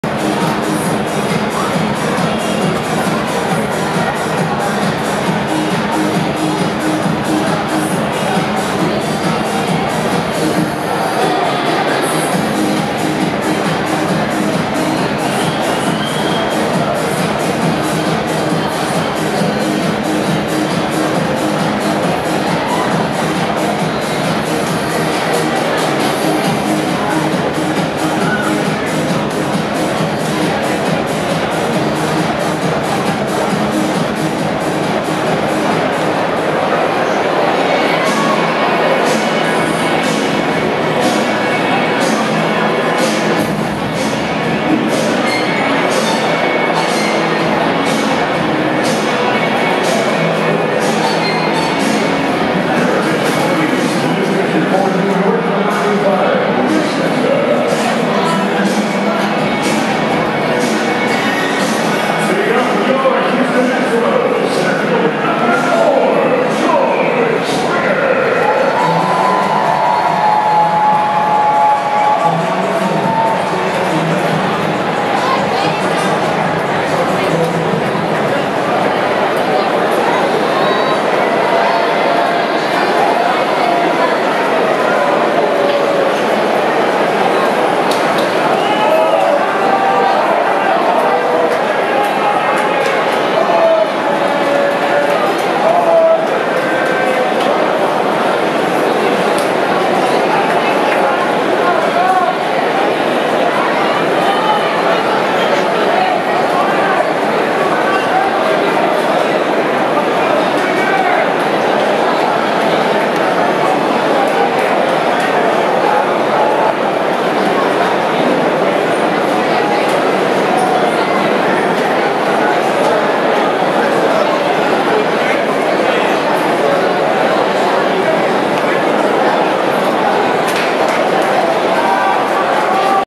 Luis Cessa pitching....Yankees vs. Astros...4/9/19
Luis Cessa faces the Astros at Minute Maid Park in Houston.